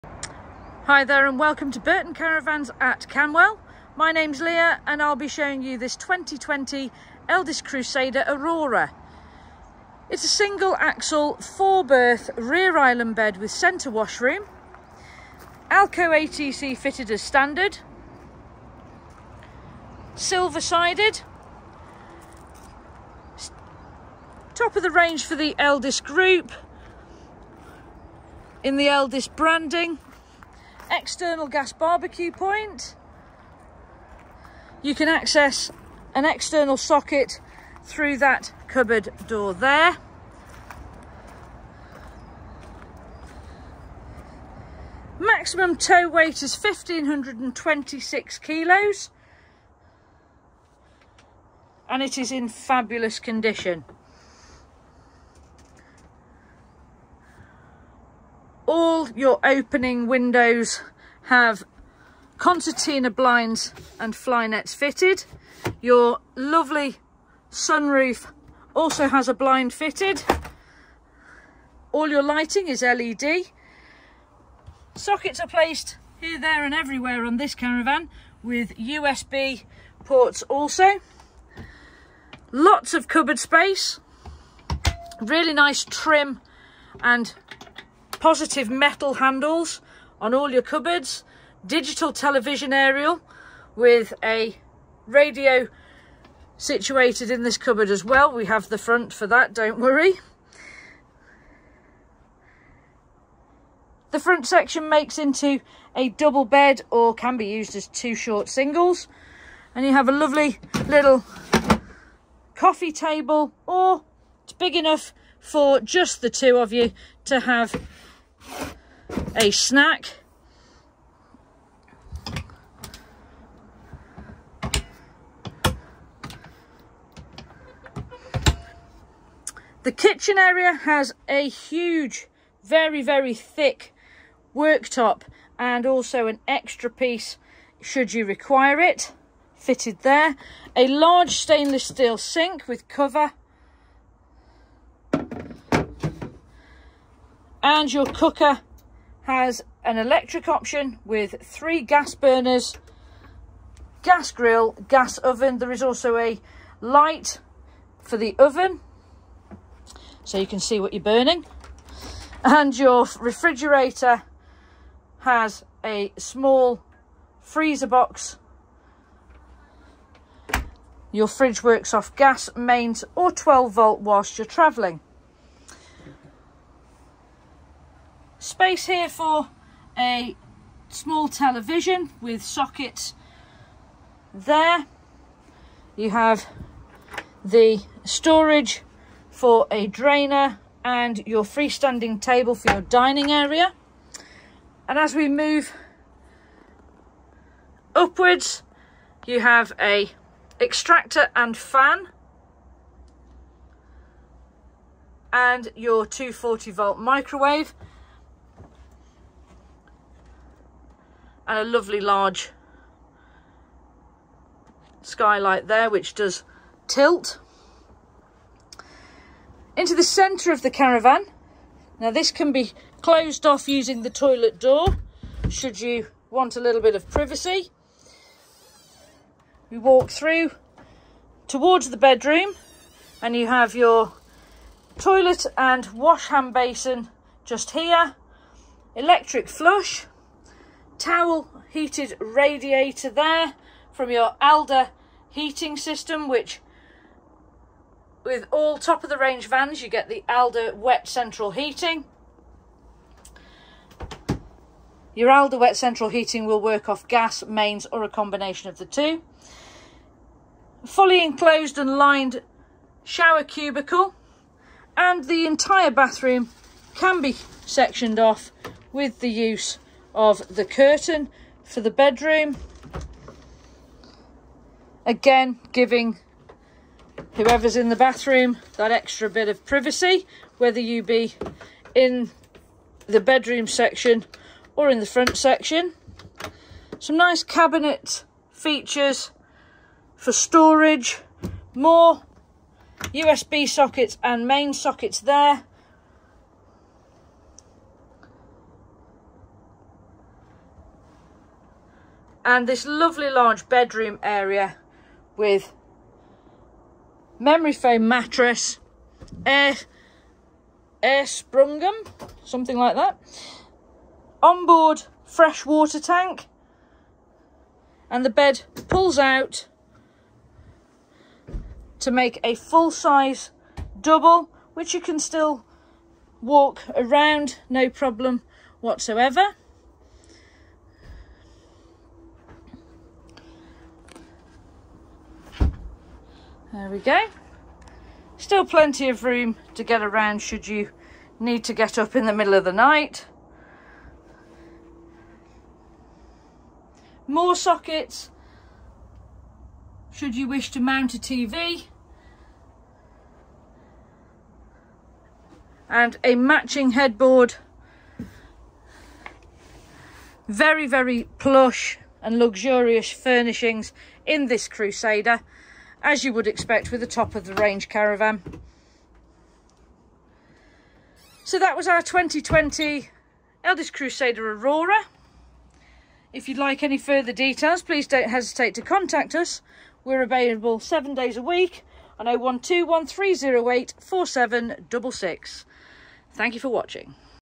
2020 Elddis Crusader Aurora
2020 Elddis Crusader Aurora
4 berth
Rear island bed centre washroom
Alde wet central heating
1526kg max tow weight
2.35m wide
7.32m overall length
£22895.00

All caravans come fully serviced, CRiS checked, demonstrated, valeted and with warranty!

BURTON CARAVANS @CANWELL
A38 LONDON ROAD
CANWELL
SUTTON COLDFIELD
B75 5ST